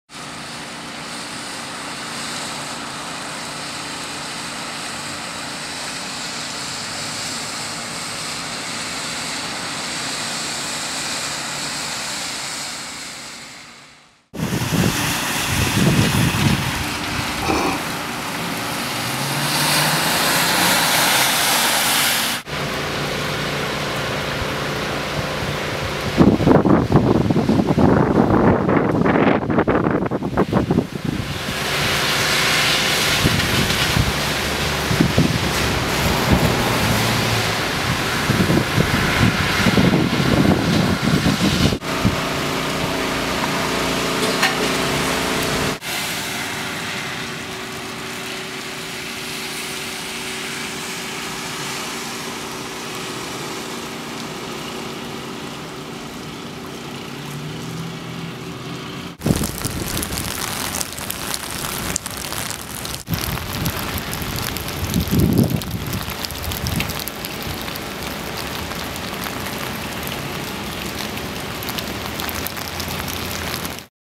Peabody MA - Floodwatch 2010
As it has been before, it will be again. Just like the flood of 2006 Peabody MA is getting it again and like before it is still amazing what 10 inches of rain will do!